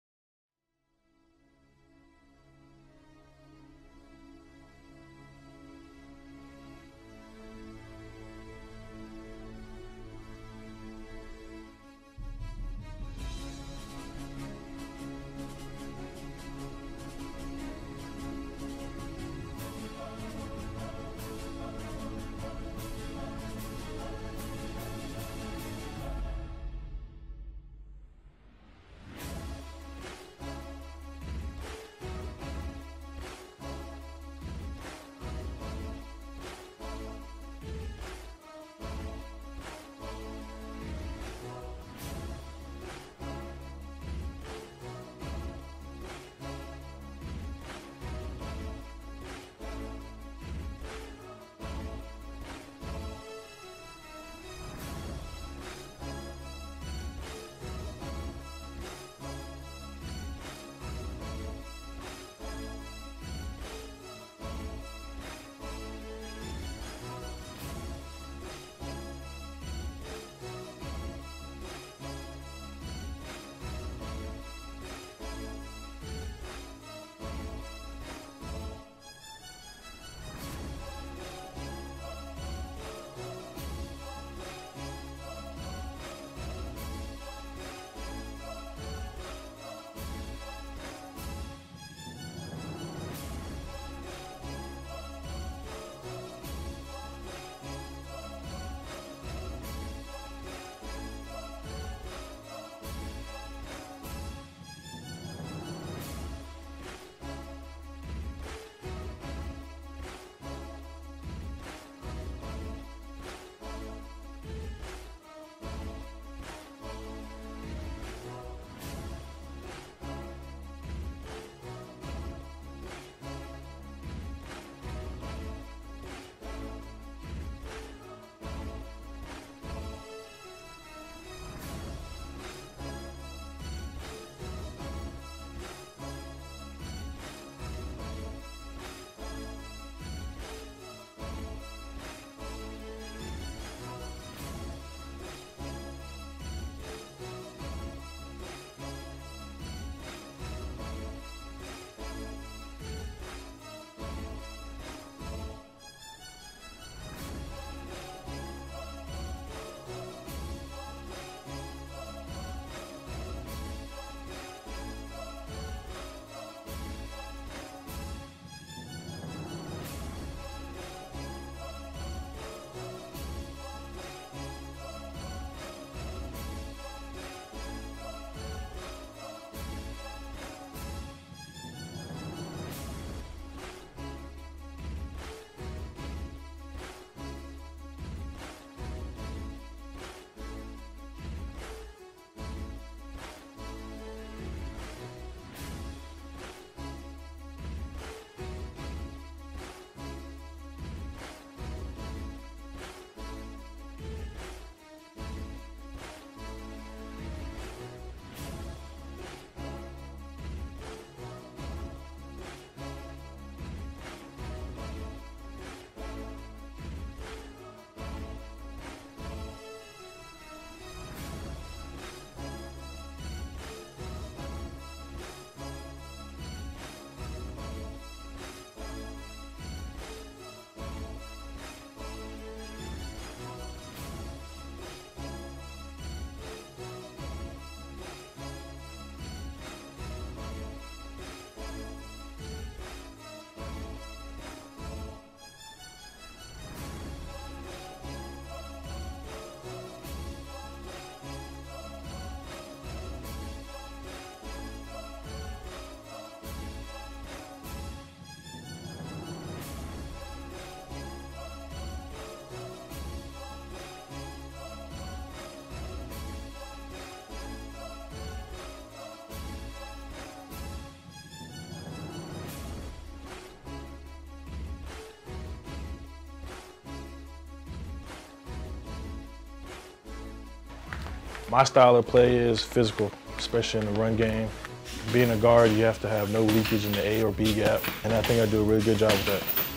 Jon Halapio - Guard | Patriots Pre-Season Highlights
Jon Halapio, former guard for the Florida Gators, was selected in the 6th round by the New England Patriots and played in 3 pre-season games. In the video above, you get a glimpse of the potential that Halapio has.

Also be sure to check out Halapio's workout & interview prior to the draft.